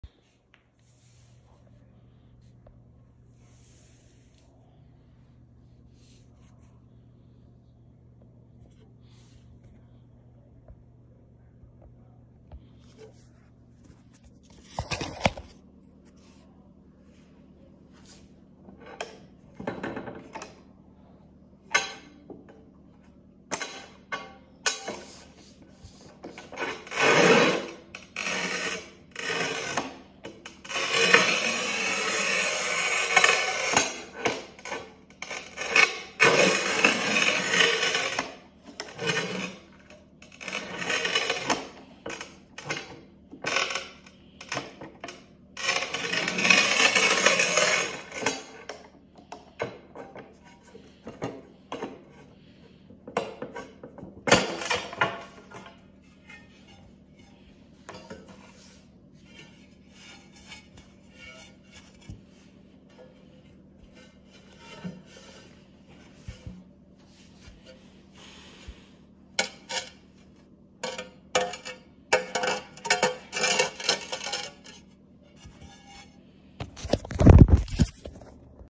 QEP PRO 21 tile cutter demo
Using my new QEP PRO 21 tile cutter, scores are clean and break is precisely straight.